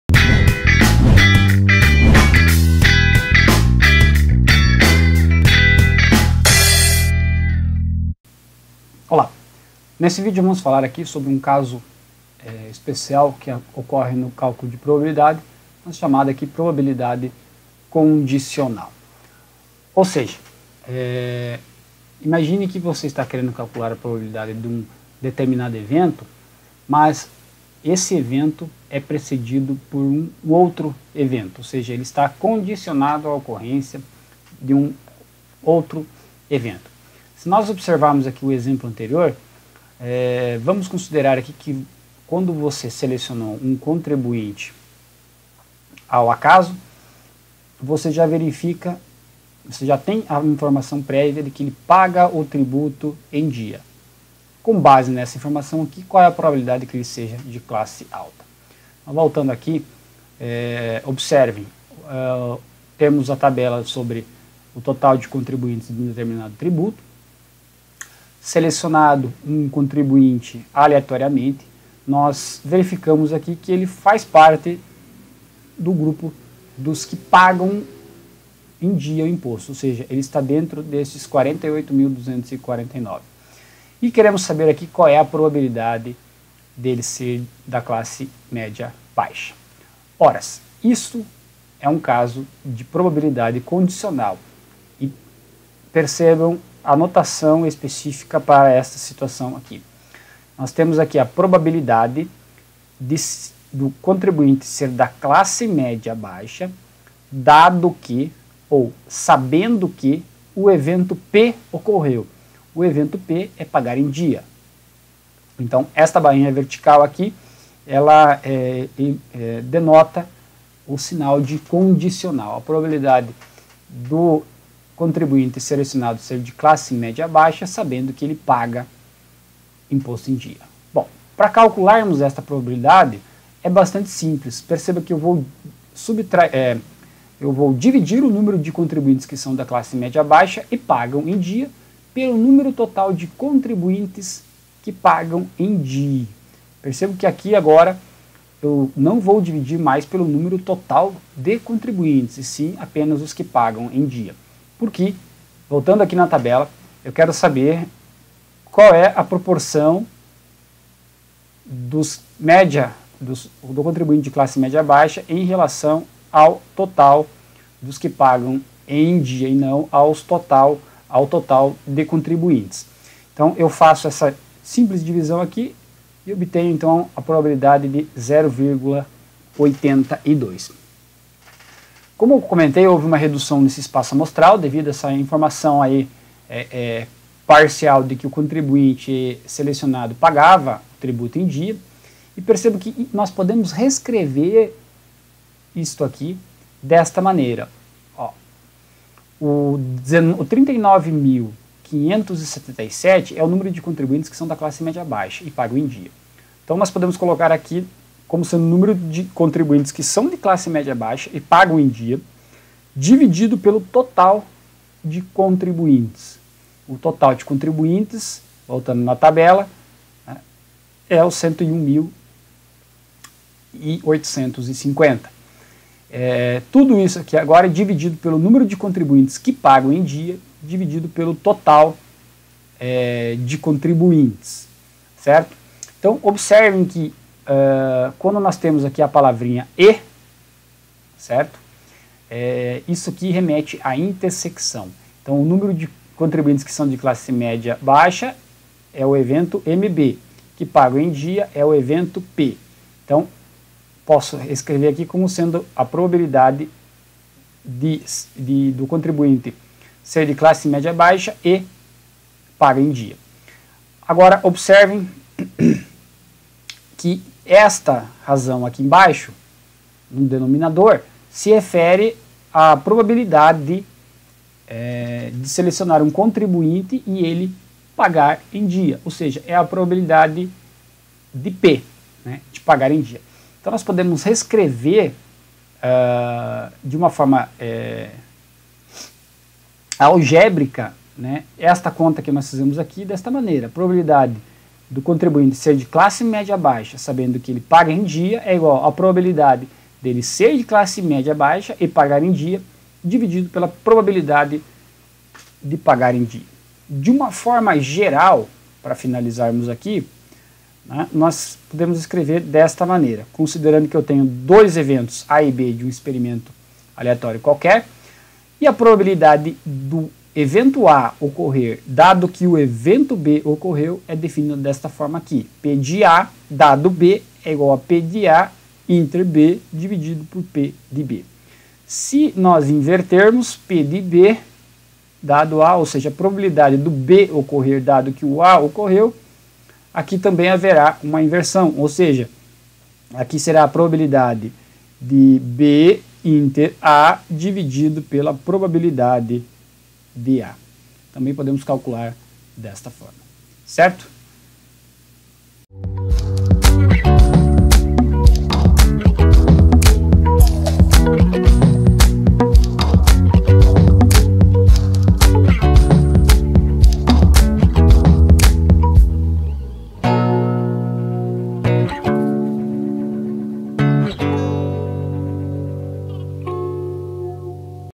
Estatística Aplicada à Administração - Probabilidade Condicional - 6
[ADMP] -   Probabilidade Condicional na disciplina de Estatística Aplicada à Administração com o professor Rudinei Bogo para o curso de Administração Pública - EaD da Universidade Estadual do Centro Oeste do Paraná.